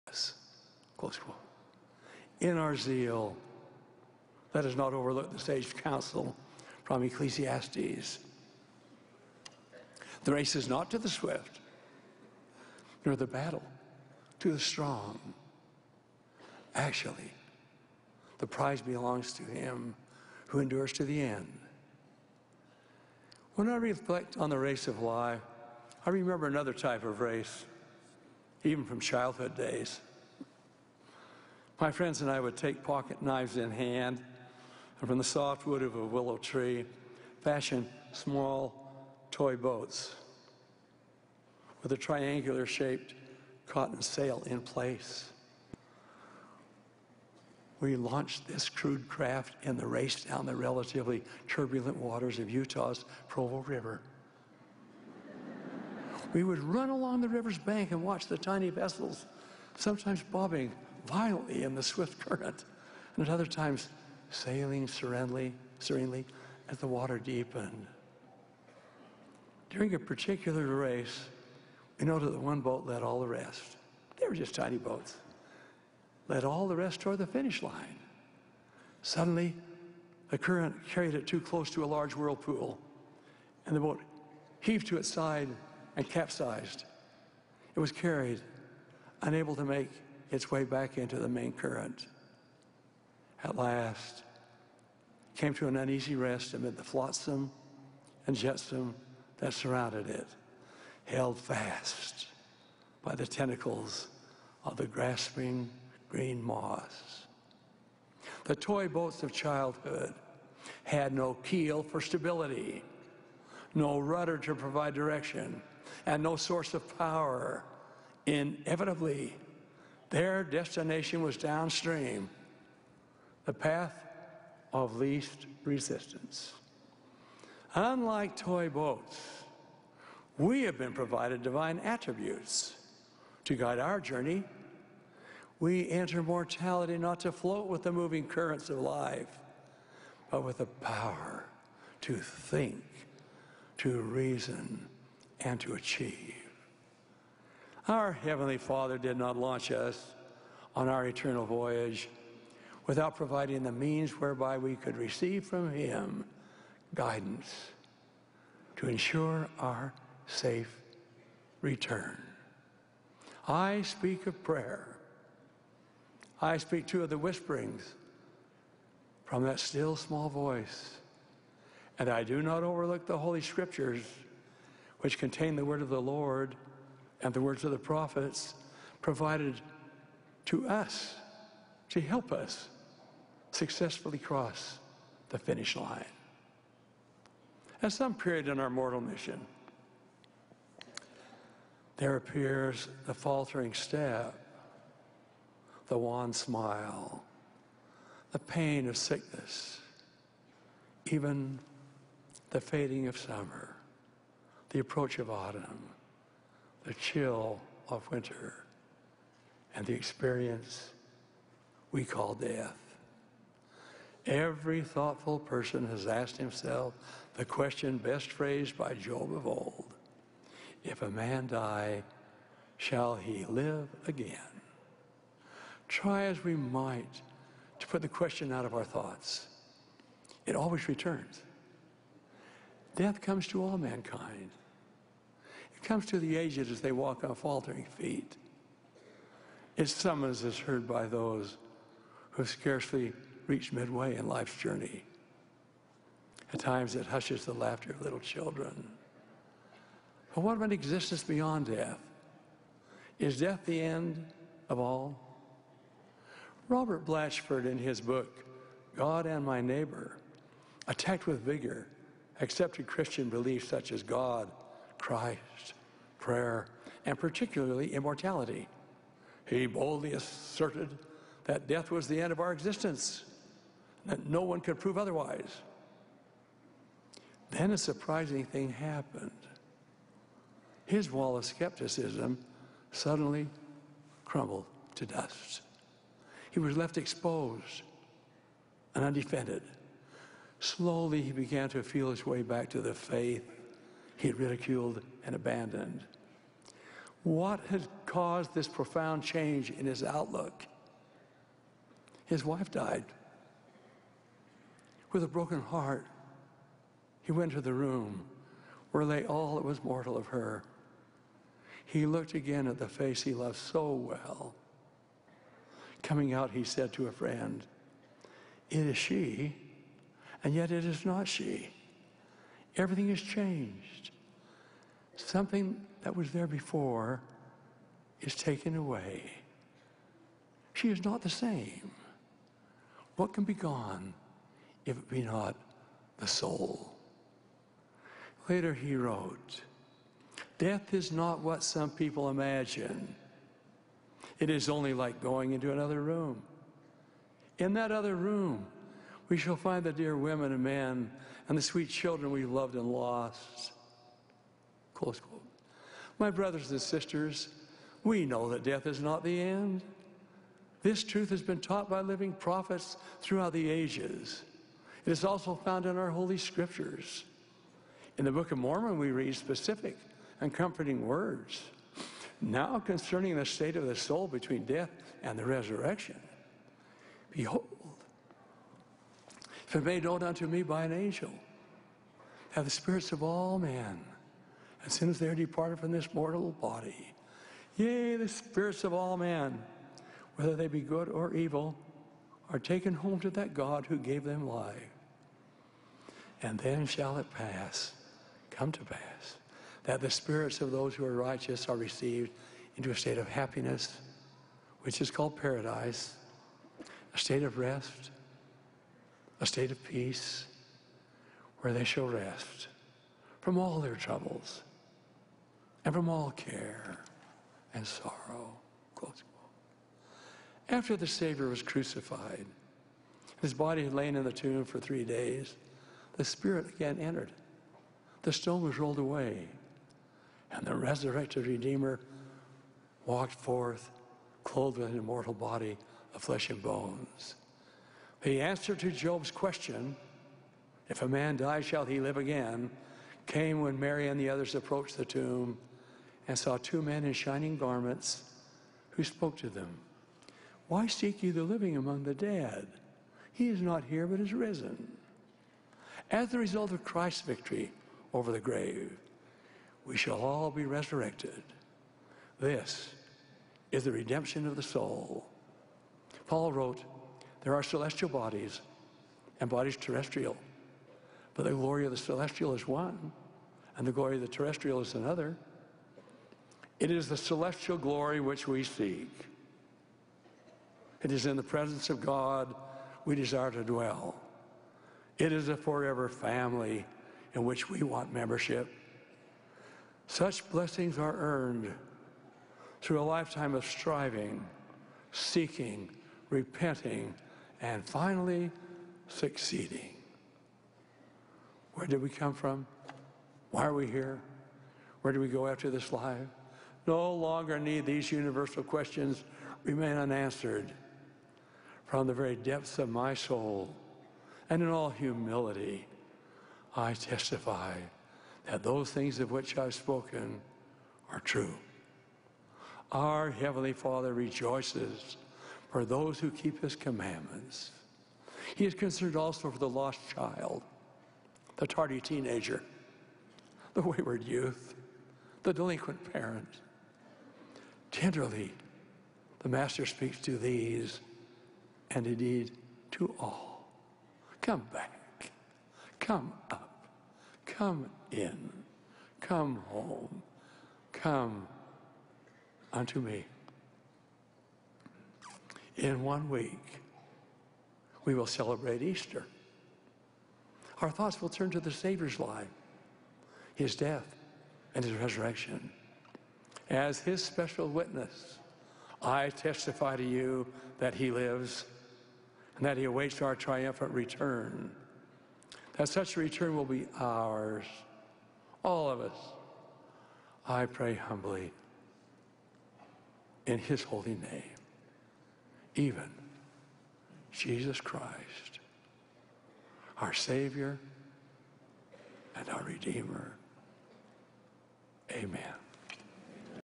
President Thomas S. Monson: The Race of Life @ 182nd LDS General Conference P2
President Thomas S. Monson: The Race of Life from the 182nd Annual General Conference of The Church of Jesus Christ of Latter-day Saints. Part 2.